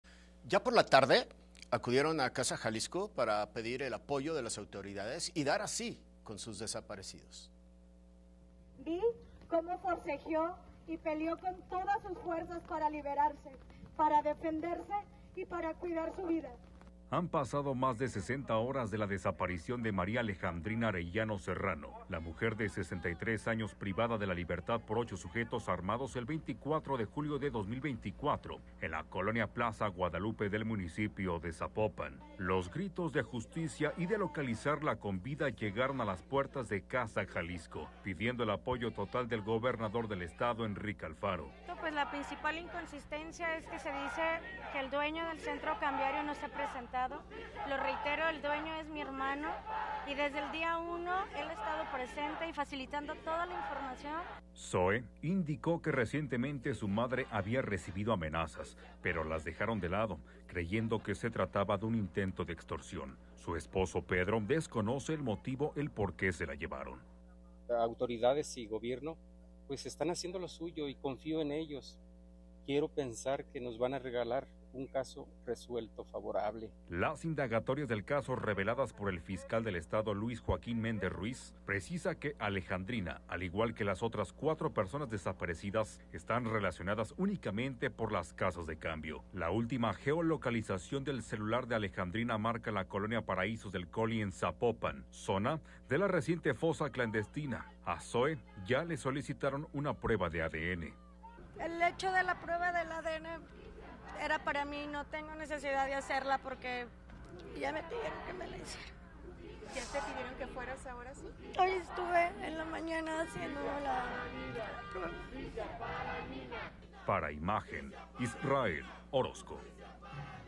Manifestación en Casa Jalisco para localizar a desaparecidos | Noticias GDL con Rey Suárez
Se manifiestan en Casa Jalisco para dar con la localización de sus familiares.

EN VIVO FB: 📰📺EnVivo Imagen Noticias GDL tercera emisión con ReySuárez